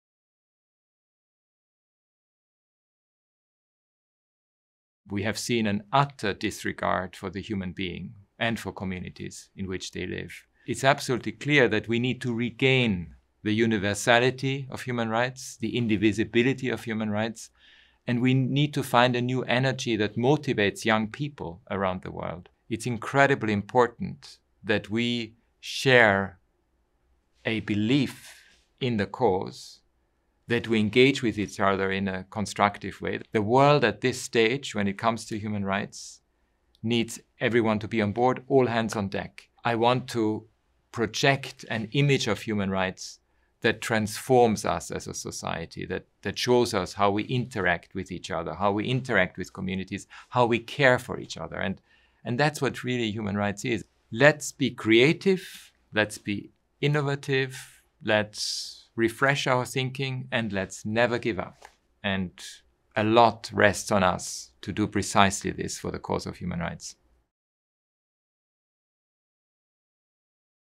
Meet our new High Commissioner Volker Türk
Volker Türk begins his mandate as the 8th UN High Commissioner for Human Rights, bringing his considerable experience in and commitment to human rights.